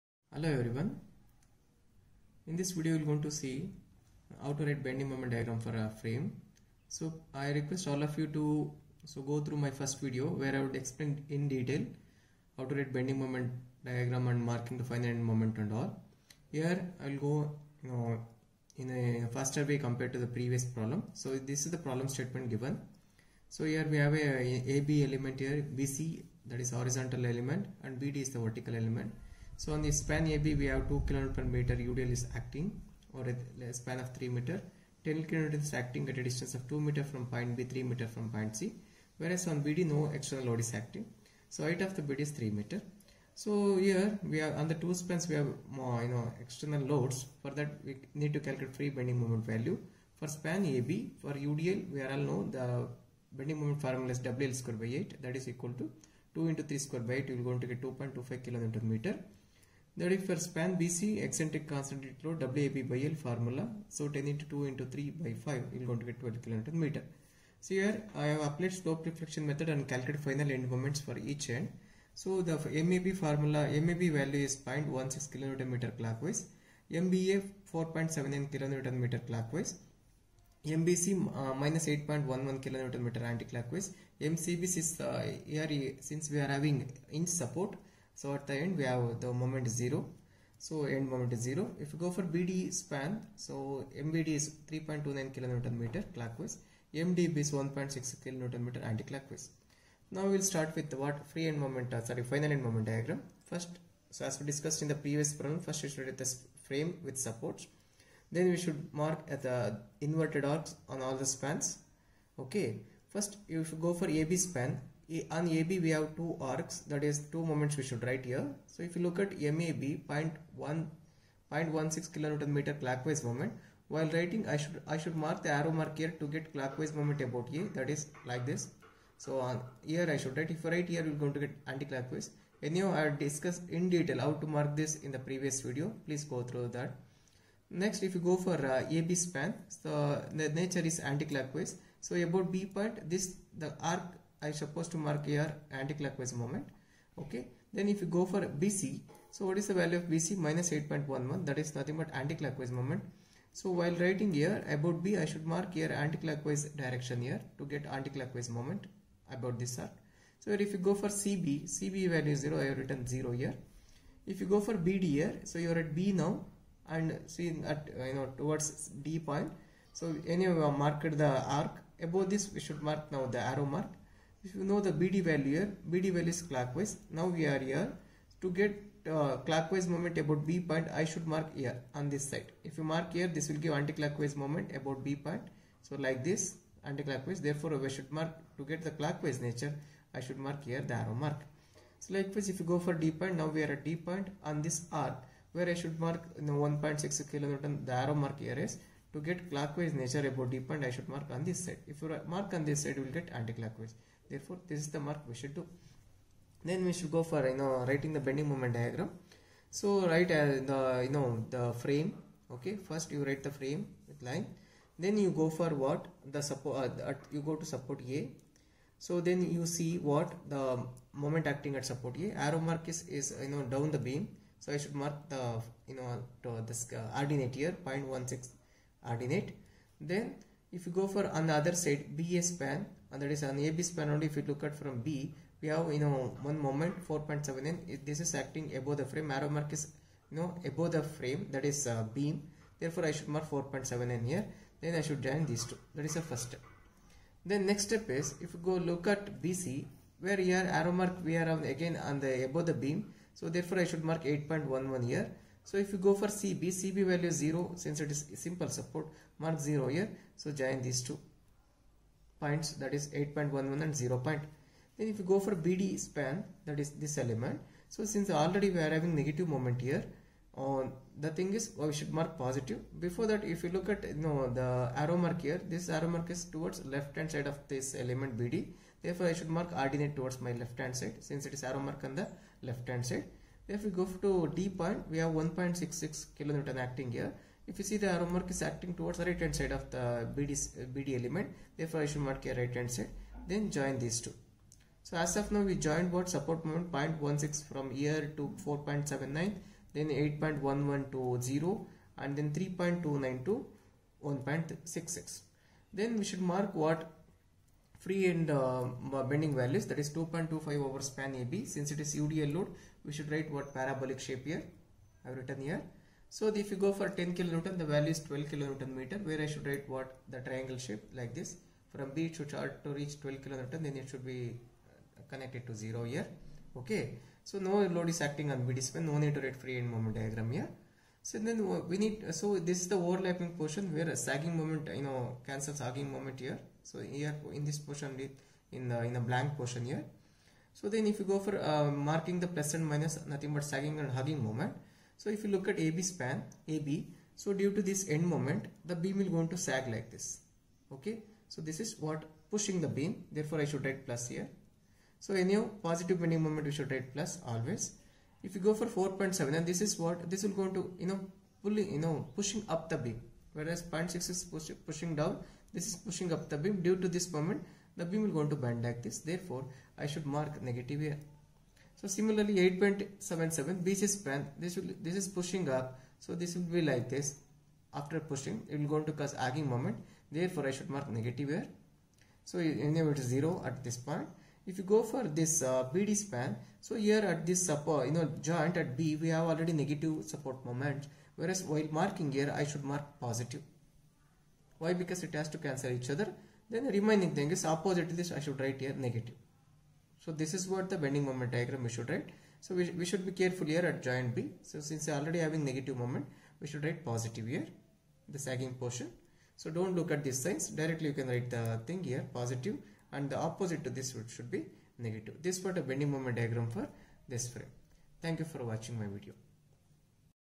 Frame Bending Moment Diagram Prob. No. 2
In this problem you will learn how to draw BMD for frame in a more simple way.